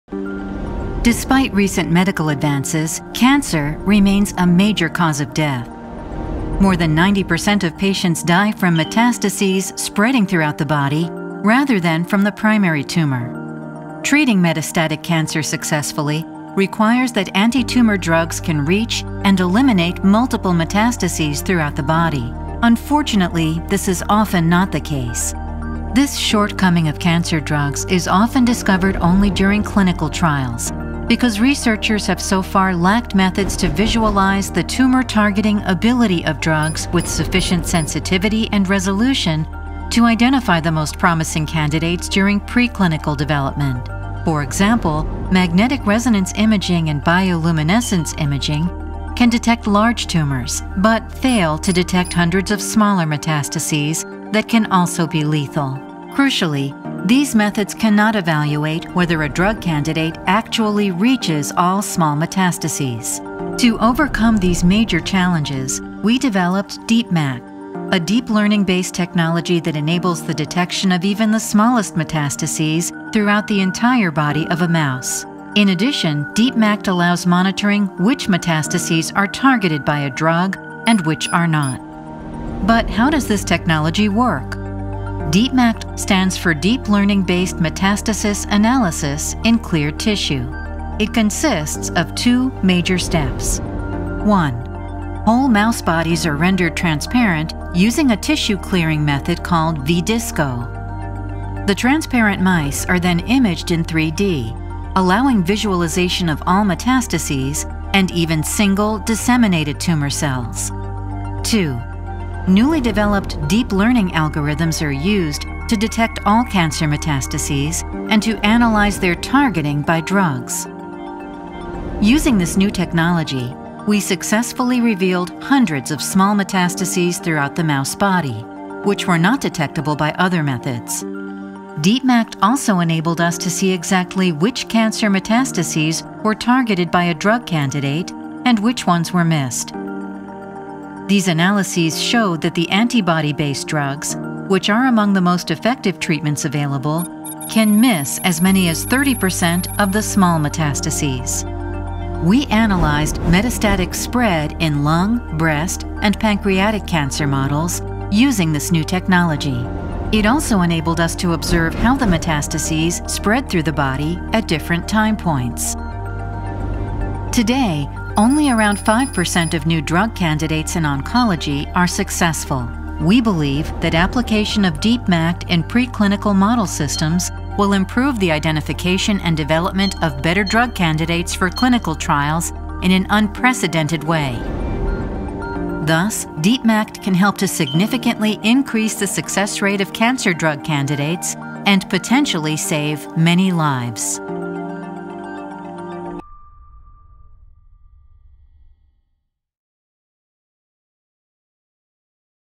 Exposing the Enemy - AI Detection of Metastases
AI enables automated detection of metastases at the level of single disseminated cancer cells in whole mice. A team at Helmholtz Zentrum München, LMU Munich and the TU of Munich have developed a new algorithm. Together with the imaging technology vDISCO the 'enemy´ metastases can be detected faster.